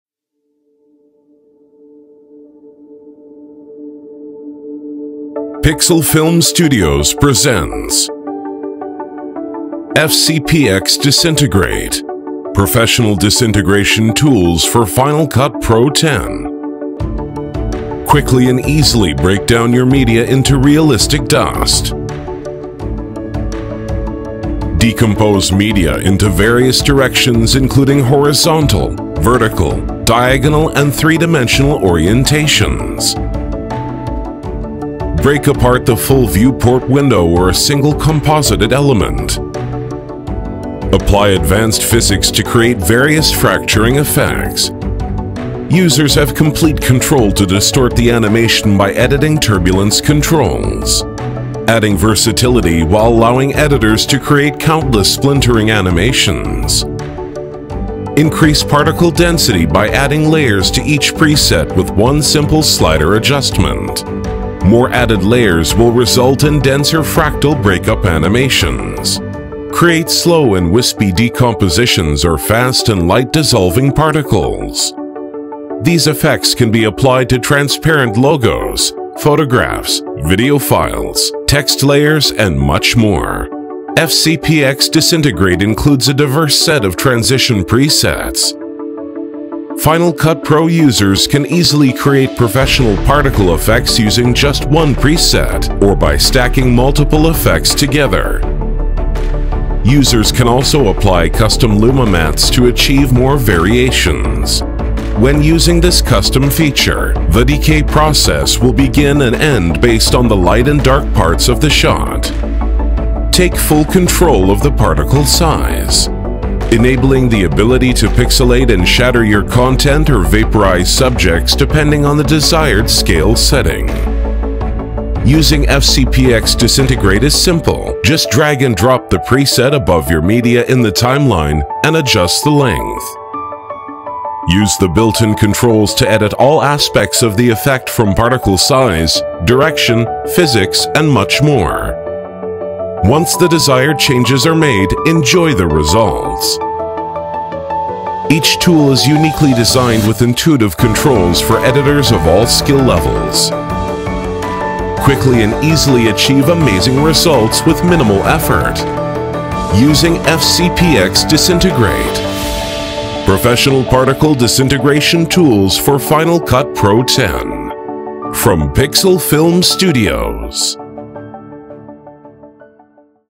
FCPX Disintegrate - Particle Disintegration Effects for FCPX - Pixel Film Studios
Final Cut Pro users can now create amazing disintegration effects with FCPX Disintegration.

Particle Disintegration Effects for FCPX - 00:20

FCPX Disintegrate is ready to dissolve or vaporize any subject in the scene. Use these professional disintegration tools to make any subject in the scene disappear or reappear. Disintegrate works great with all types of footage and subject types, use it with logos, full screen media, text layers, and green screen footage.

FCPX Disintegrate Tools - 02:15

FCPX Disintegrate is a unique tool designed for Final Cut Pro users of all skill levels. This plugin includes both title layer presets and transition presets. Easily add dissolve and disintegration effects to any media type. Users have full control over particle physics and custom luma mattes.

Title Layers - 02:04

FCPX Disintegrate includes a basic tool, and a more advanced tool for those that want to take their animation to the next level. Simply place the preset over your media in the timeline and edit the particle disintegration controls to your liking.

Transitions - 01:24

This plugin includes 6 different particle dissolve transitions, each with easy to use controls and ultimate customizability.

Any Media Type - 01:16

FCPX Disintegrate is extremely versatile, it can be used to dissolve text layers, company logos, images, videos and much more.

Any Resolution/Media Types

FCPX Disintegrate is compatible with all resolutions and aspect ratios including HD, 4K, 5K, and mobile. These tools work with all video types making it extremely flexible for any project.

Browse our site for even more incredible Final Cut Pro X plugins!